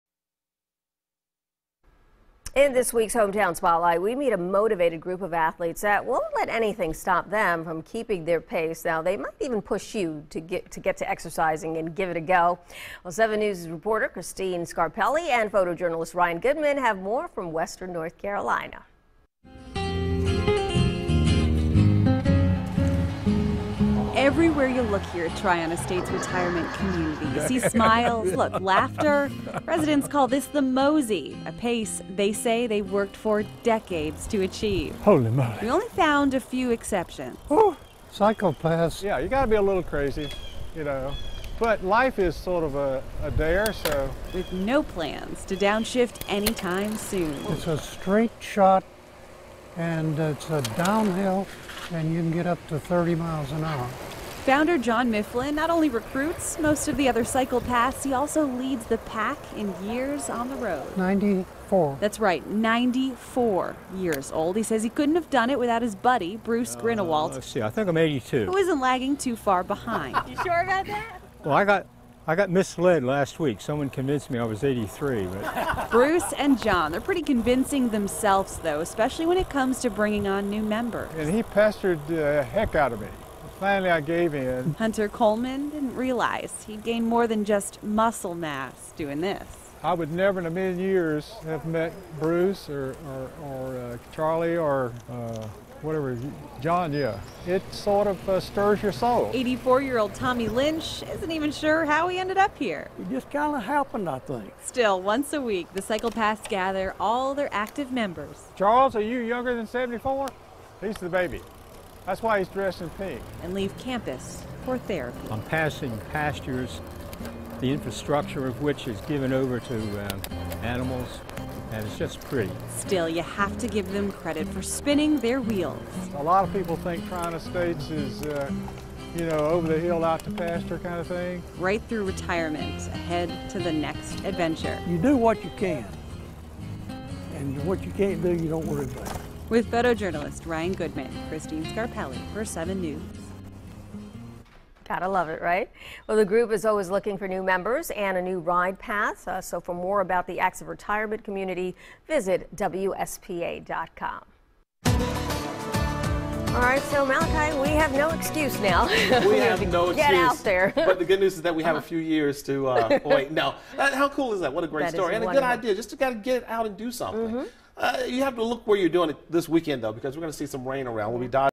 Hometown Spotlight with the Cyclepaths
This week for Hometown Spotlight we're in a retirement community where the older adults just won't slow down. We've got so much to show you including a group of cyclists remaining athletic into their 90's.